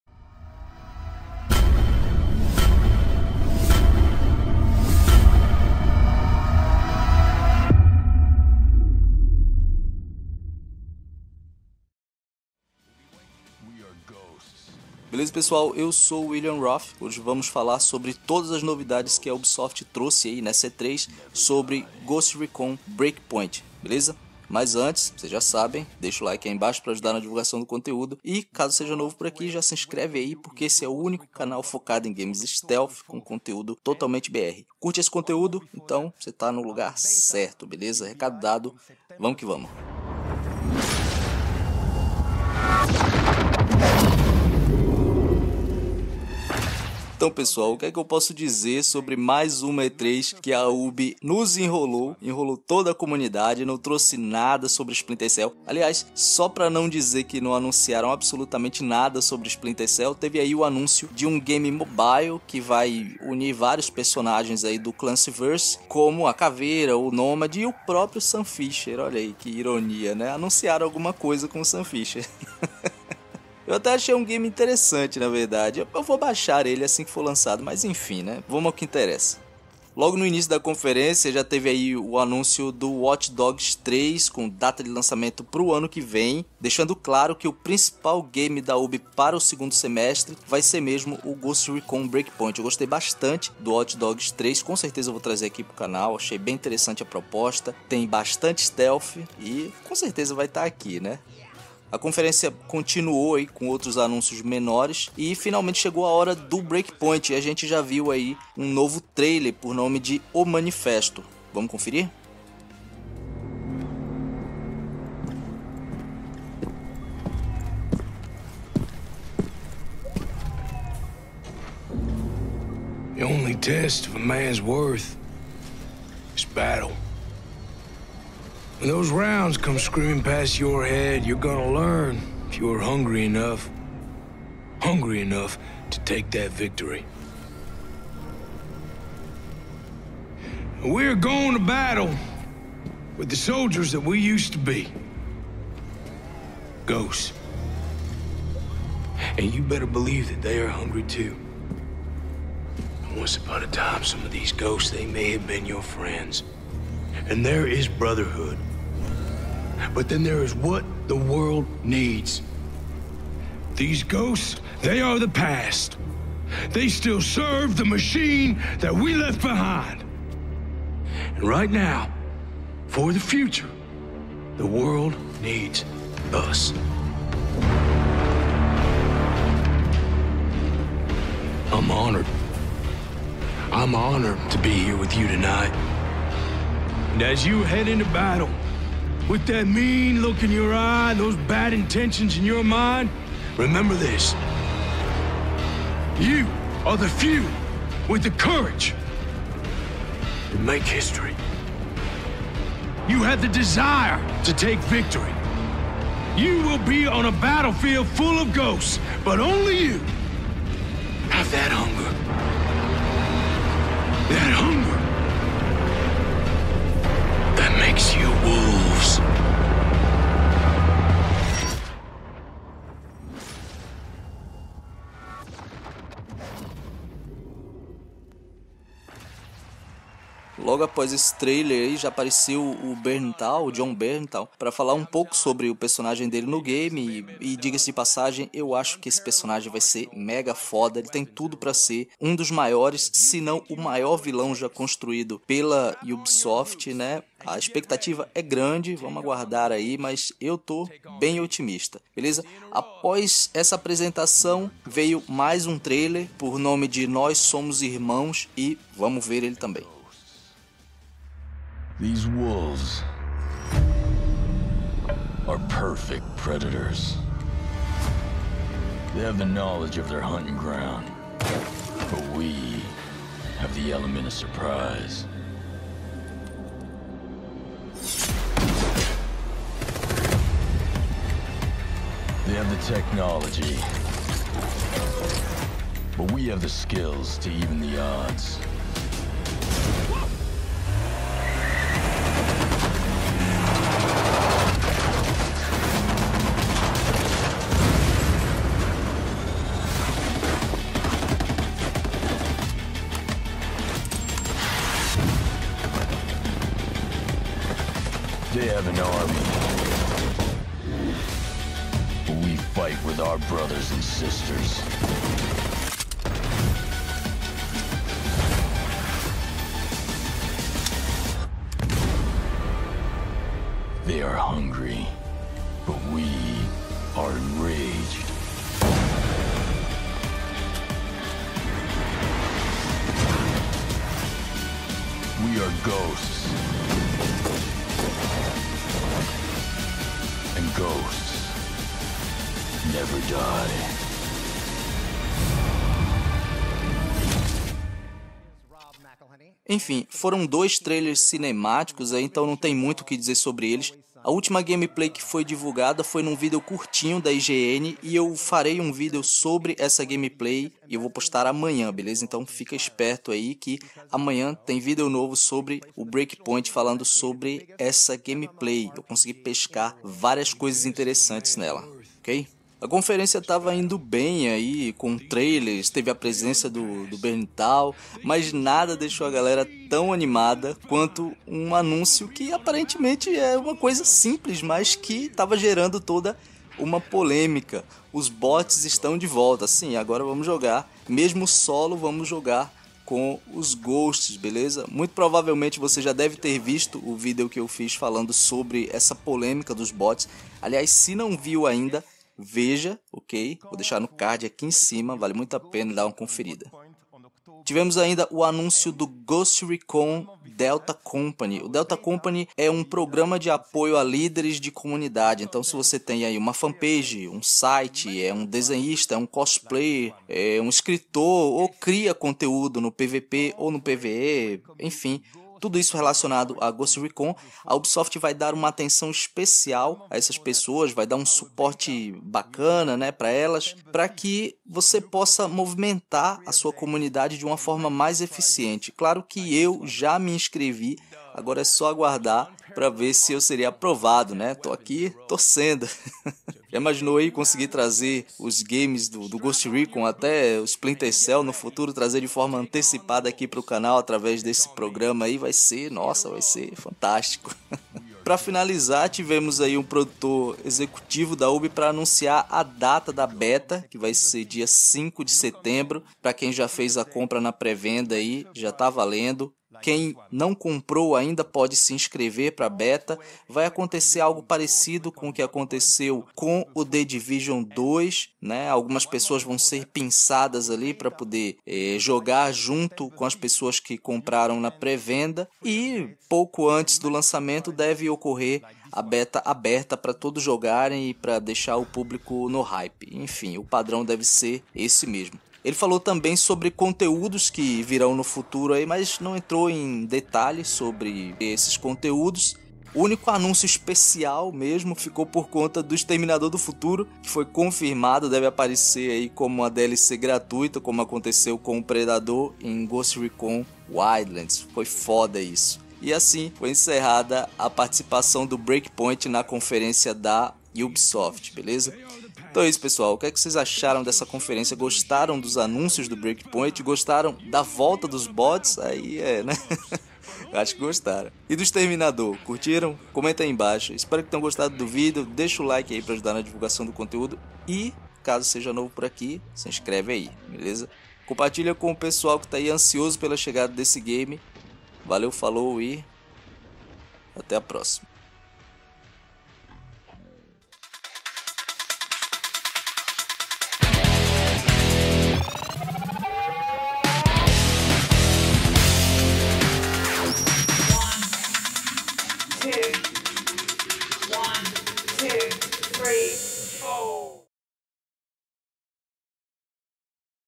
Grandes novidades em Ghost Recon Breakpoint
Fala Pessoal! Hoje vamos conversar um pouco sobre o novo Tom Clancy Ghost Recon Breakpoint que foi anunciado dia 9 de maio de 2019 e as grandes novidades anunciadas na E3 2019.

PSN: willianrof